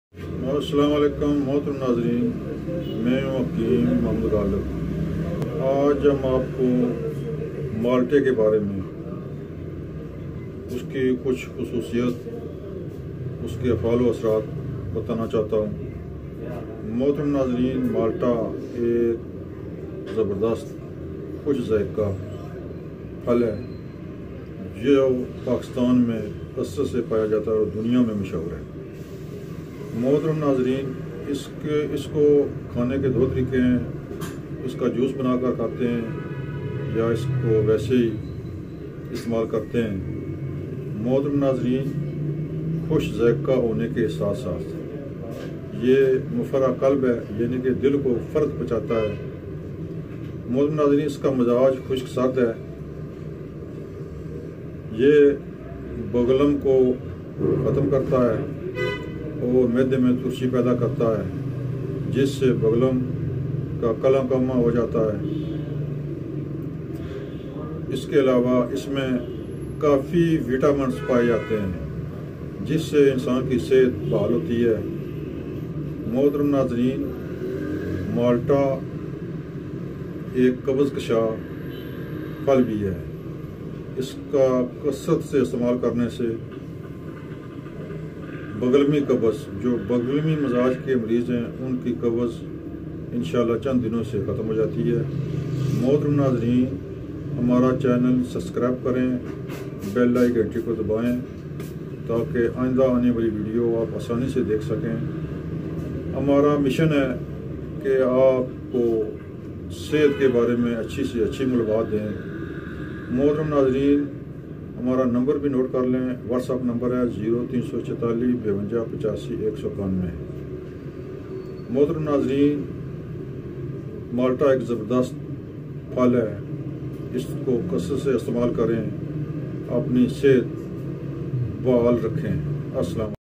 مالٹے کثرت سے استعمال کریں چہرے بنایئں لال سرخ مالٹے سے قبض کا زبردست علاج malta
مالٹا  ذائقہ  دار  گھٹا  میٹھا  پھل